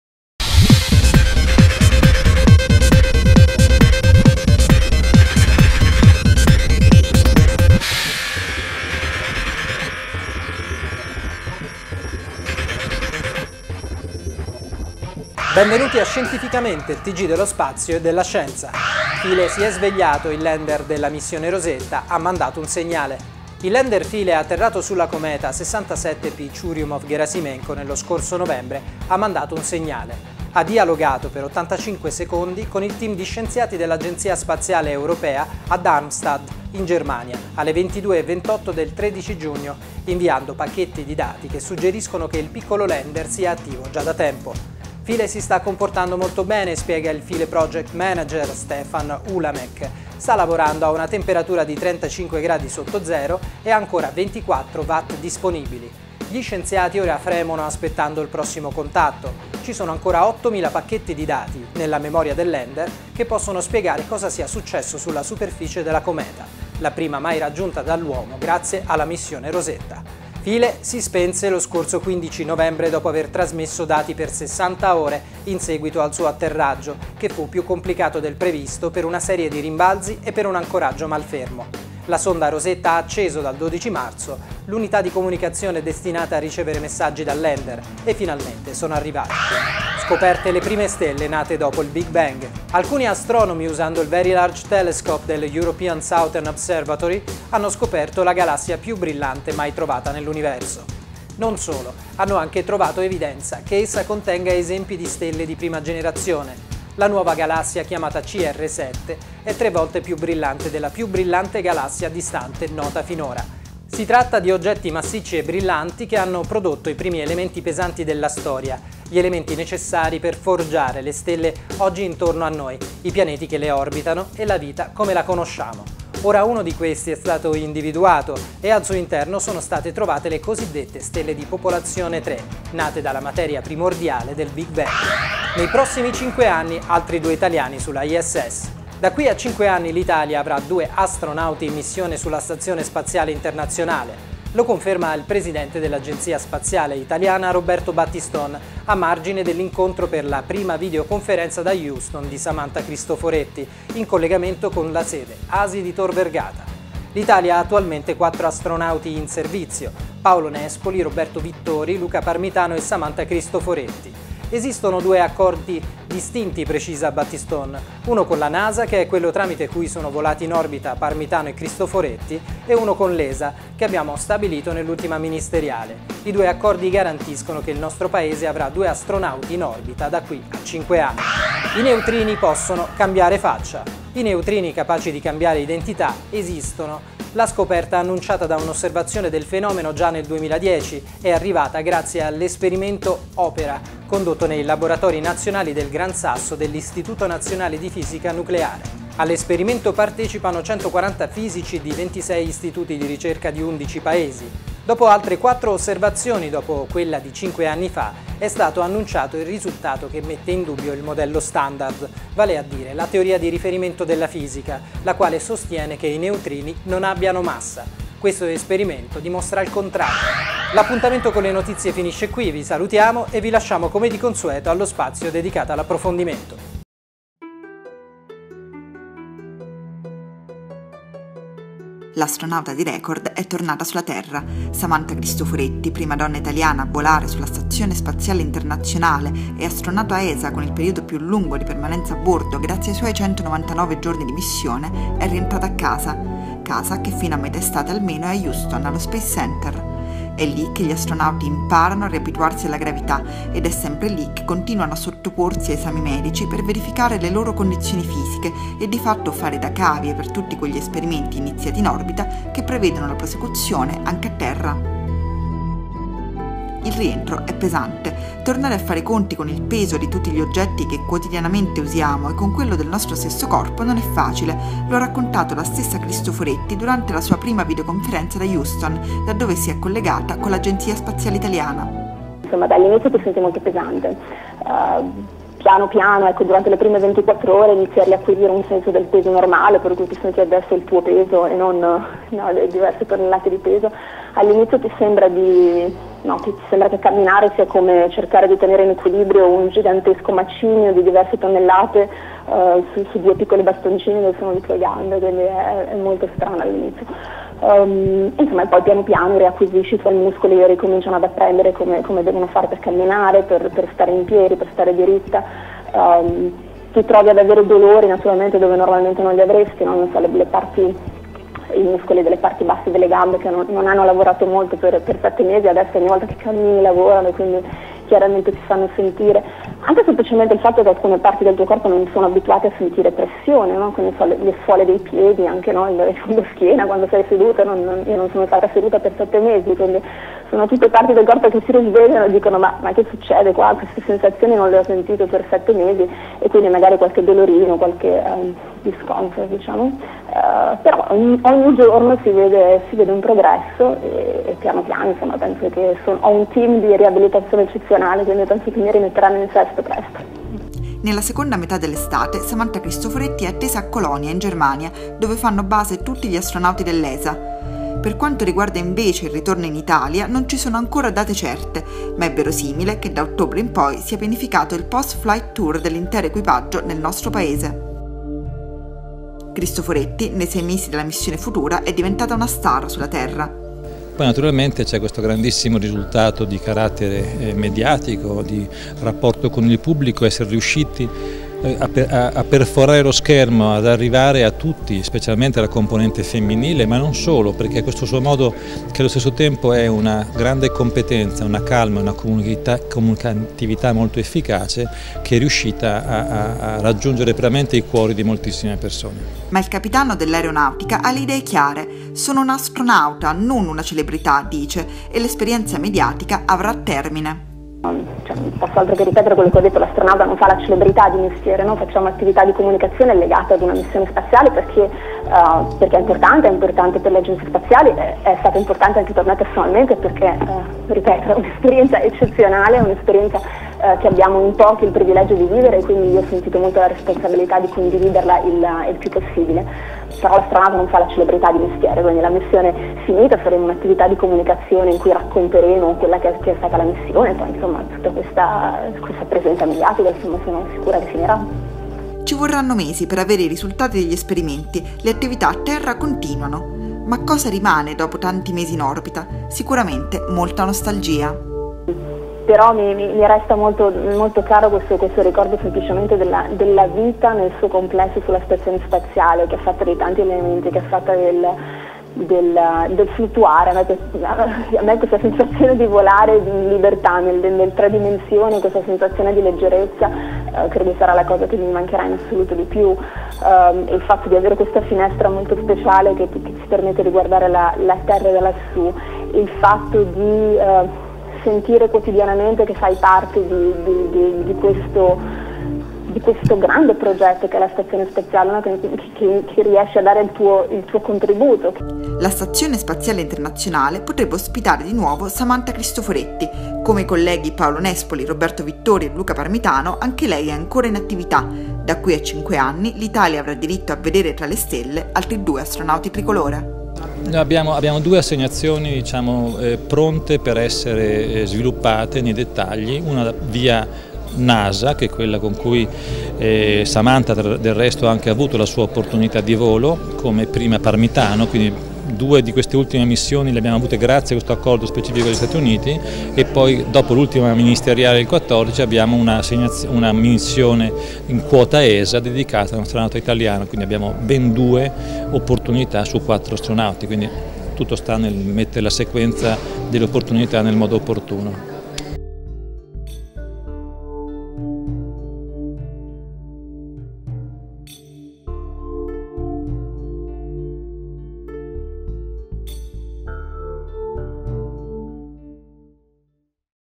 La “nuova” vita sulla Terra di Samantha Cristoforetti
L'astronauta dei record è tornata sulla Terra. Nella seconda metà dell'estate è attesa a Colonia, in Germania e in ottobre il post flight tour in Italia

Per saperne di più e rimanere aggiornato, clicca quì

Nelle NEWS:
- PHILAE SI È SVEGLIATO, IL LANDER DELLA MISSIONE ROSETTA HA MANDATO UN SEGNALE
- SCOPERTE LE PRIME STELLE NATE DOPO IL BIG BANG
- NEI PROSSIMI 5 ANNI ALTRI DUE ITALIANI SULLA ISS
- I NEUTRINI POSSONO CAMBIARE 'FACCIA'